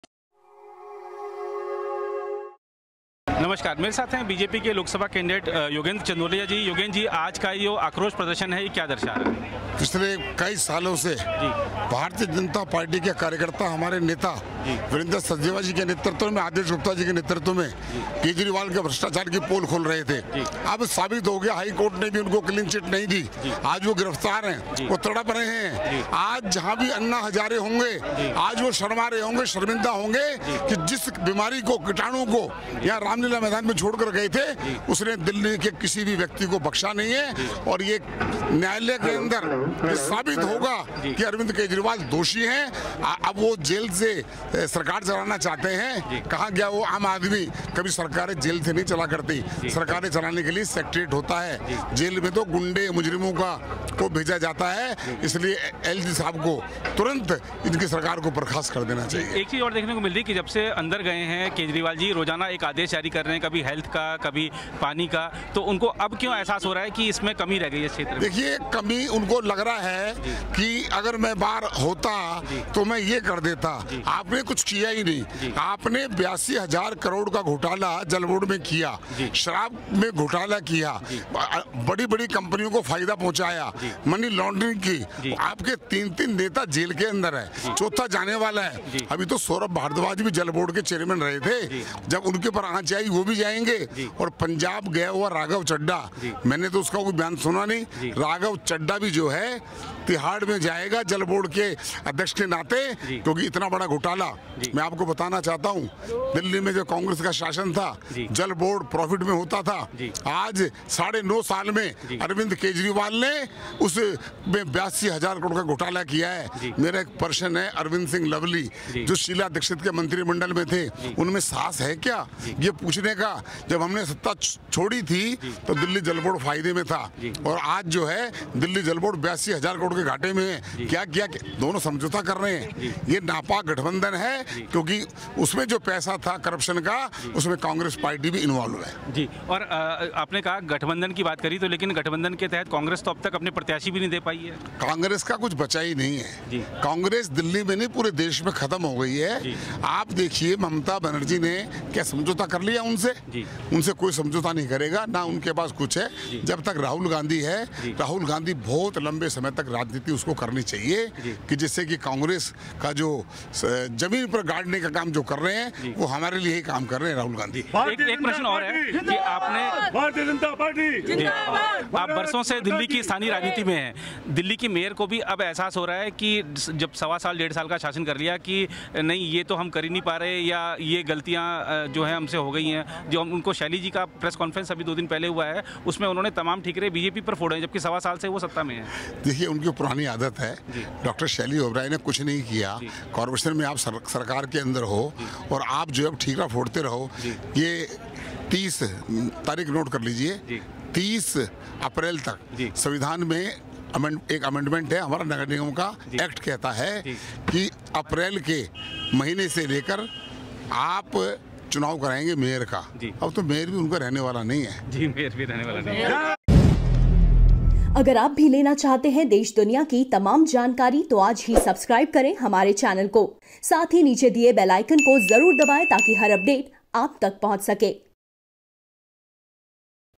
Yogendra Chandolia ने गिनाए Kejriwal सरकार के घोटाले, MCD में सत्तारुढ़ AAP पर भी साधा जोरदार निशाना
Loksabha Elections में Delhi से BJP Candidate Yogendra Chandolia ने Arvind Kejriwal सरकार पर निशाना साधते हुए कहा कि यह सरकार जन विश्वास को खो चुकी है।

संबंधित अन्य विस्तृत समाचार पढ़ने के लिए क्लिक करें

0:00 एंकर
0:10 योगेंद्र चंदोलिया से बातचीत